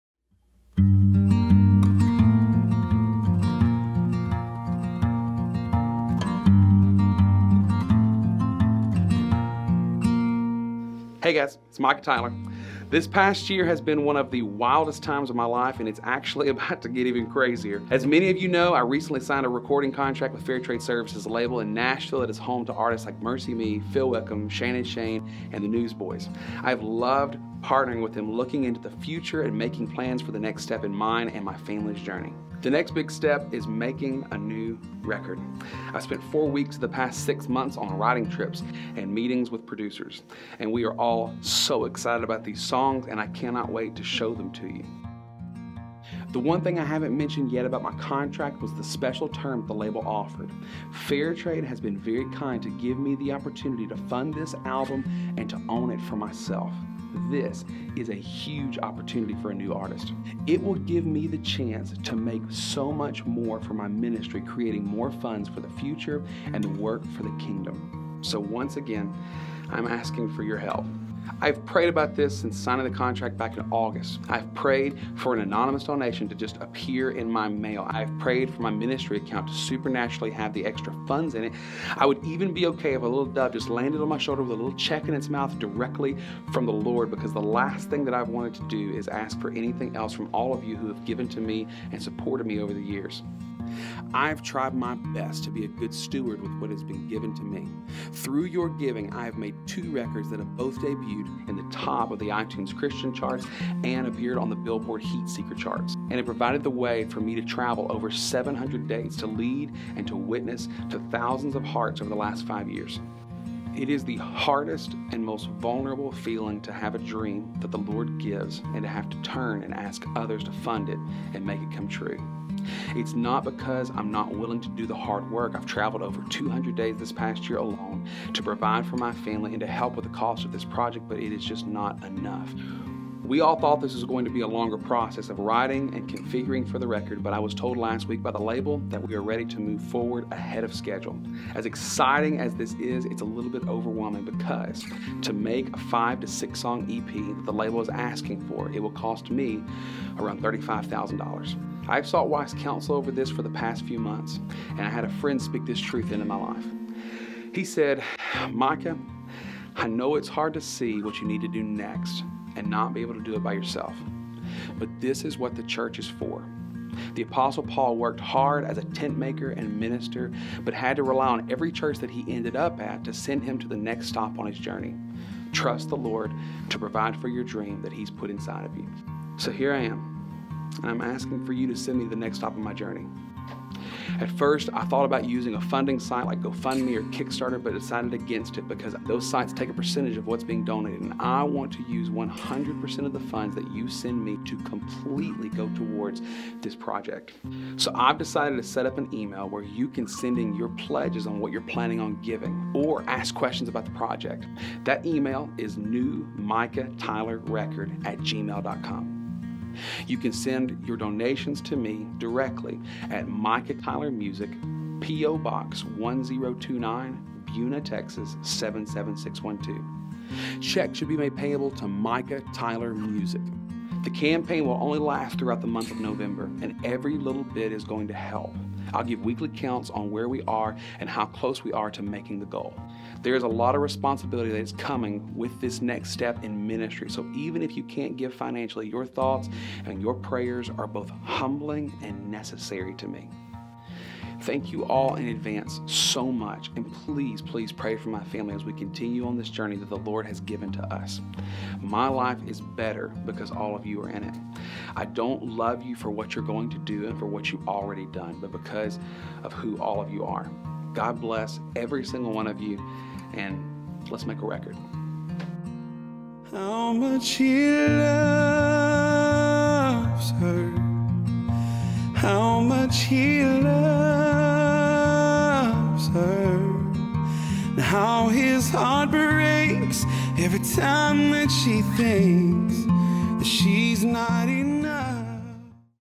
New Micah Tyler Record
Help Micah make his debut album with Fair Trade Services! Watch the video and pray about how you can help.

Checks made payable:
"Micah Tyler Music"